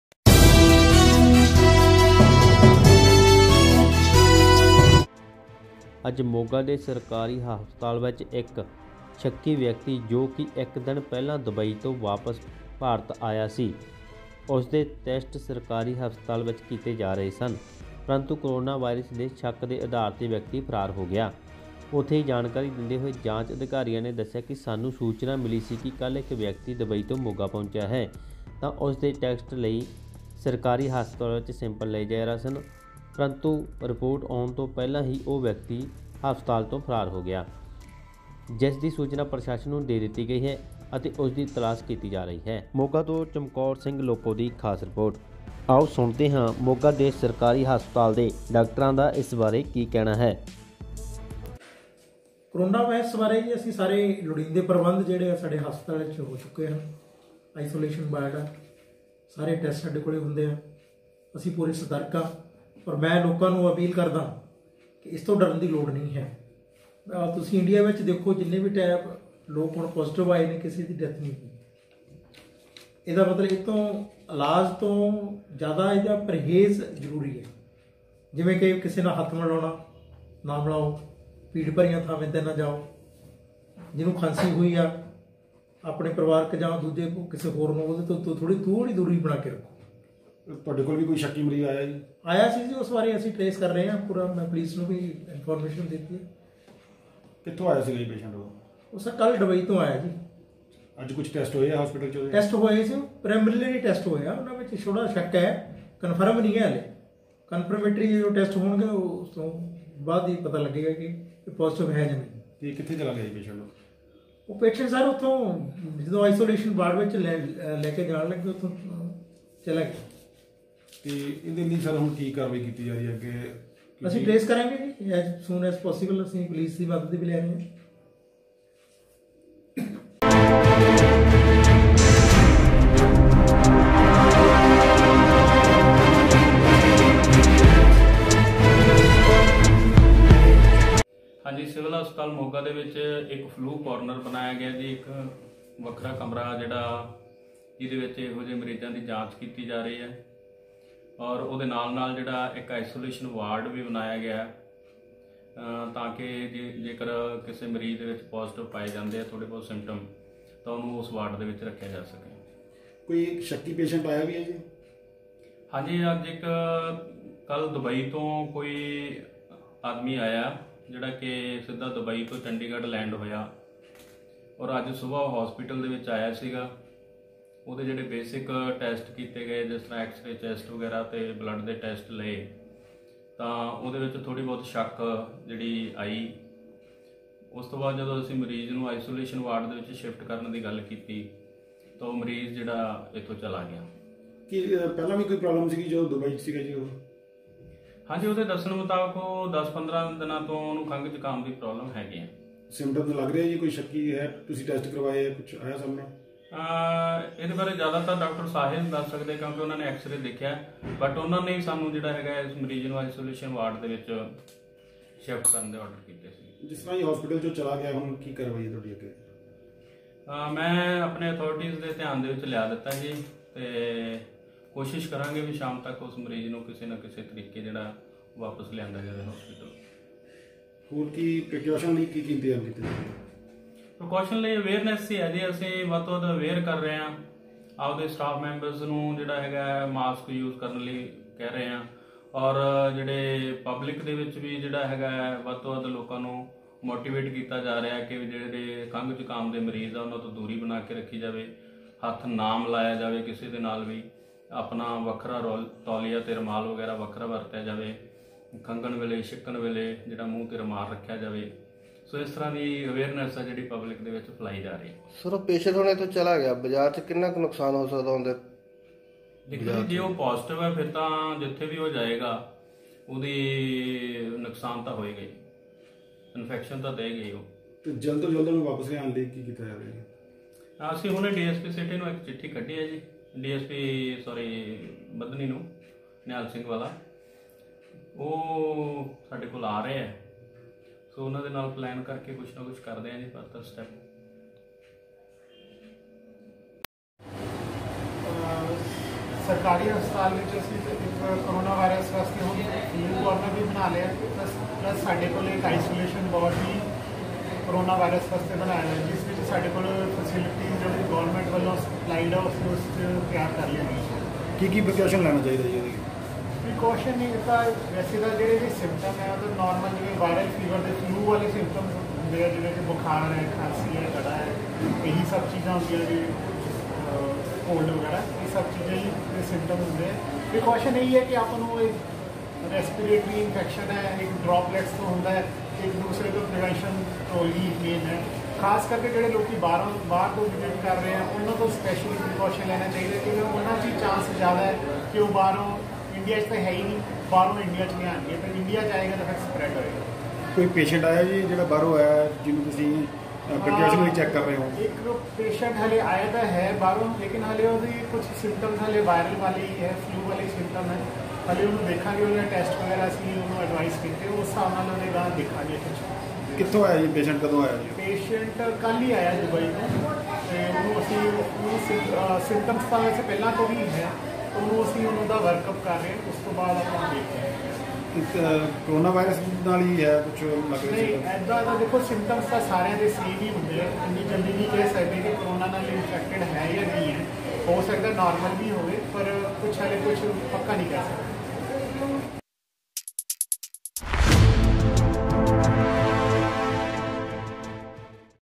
breaking news / ਮੋਗਾ ਵਿੱਚ ਕਾਰੋਨਾ ਵਾਇਰਸ ਬਾਰੇ ਦੇਖੋ ਪੂਰੀ ਖਬਰ /news Punjab di
ਅੱਜ ਮੋਗਾ ਦੇ ਸਰਕਾਰੀ ਹਸਪਤਾਲ ਅੱਜ ਇੱਕ ਸ਼ੱਕੀ ਵਿਅਕਤੀ ਜੋ ਕਿ ਇੱਕ ਦਿਨ ਪਹਿਲਾਂ ਦੁਬਈ ਤੋਂ ਵਾਪਸ ਭਾਰਤ ਆਇਆ ਸੀ ਉਸਦੇ ਟੈਸਟ ਮੋਗਾ ਦੇ ਸਰਕਾਰੀ ਹਸਪਤਾਲ ਵਿੱਚ ਕੀਤੇ ਜਾ ਰਹੇ ਹਨ ਪ੍ਰੰਤੂ ਕਰੋਨਾ ਵਾਇਰਸ ਦੇ ਸ਼ੱਕ ਦੇ ਆਧਾਰ ਤੇ ਵਿਅਕਤੀ ਫਰਾਰ ਹੋ ਗਿਆ ਉਥੇ ਹੀ ਜਾਣਕਾਰੀ ਦਿੰਦੇ ਹੋਏ ਜਾਂਚ ਅਧਿਕਾਰੀਆਂ ਨੇ ਦੱਸਿਆ ਕਿ  ਸਾਨੂੰ ਸੂਚਨਾ ਮਿਲੀ ਸੀ ਕਿ ਕੱਲ੍ਹ ਇੱਕ ਵਿਅਕਤੀ ਦੁਬਈ ਤੋਂ ਮੋਗਾ ਪਹੁੰਚਿਆ ਹੈ ਤਾਂ ਉਸ ਦੇ ਟੈਕਸ  ਲਈ ਸਰਕਾਰੀ ਹਸਪਤਾਲ ਵਿੱਚ ਸੈਂਪਲ ਲਏ ਜਾ ਰਹੇ ਹਨ ਪਰੰਤੂ ਰਿਪੋਰਟ ਆਉਣ ਤੋਂ ਪਹਿਲਾਂ ਹੀ ਉਹ ਵਿਅਕਤੀ ਹਸਪਤਾਲ  ਤੋਂ ਫ਼ਰਾਰ ਹੋ ਗਿਆ ਜਿਸ ਦੀ ਸੂਚਨਾ ਪ੍ਰਸ਼ਾਸਨ ਨੂੰ ਦੇ ਦਿੱਤੀ ਗਈ ਹੈ ਅਤੇ ਉਸ ਦੀ ਤਲਾਸ਼ ਕੀਤੀ ਜਾ ਰਹੀ ਹੈ
ਇੱਥੇ ਇਹ ਵੀ ਦੱਸਣਾ ਜ਼ਰੂਰੀ ਹੈ ਕਿ ਸਿਹਤ ਵਿਭਾਗ ਨੇ ਇੱਕ ਪੋਸਟ ਰਾਹੀਂ ਮੀਡੀਆ ਨੂੰ ਵਿਅਕਤੀ ਦੀ ਪਹਿਚਾਣ ਗੁਪਤ ਰੱਖੀ ਰੱਖਣ ਲਈ ਕਿਹਾ ਹੈ ਤਾਂ ਜੋ ਉਨ੍ਹਾਂ ਦੇ ਪਰਿਵਾਰ ਨੂੰ ਕੋਈ ਵੀ ਗਲਤ ਪ੍ਰਭਾਵ ਨਾ ਪਵੇ ਕਿਉਂਕਿ ਹਾਲੇ ਸੈਂਪਲ ਹੀ ਲਏ ਨੇ ਤੇ ਵਿਅਕਤੀ ਡਰਦਾ ਹੀ ਭੱਜ ਨਿਕਲਿਆ ਅੱਜ ਕਰੋਨਾ ਵਾਇਰਸ ਦੀ ਪੁਸ਼ਟੀ ਨਹੀਂ ਹੋਈ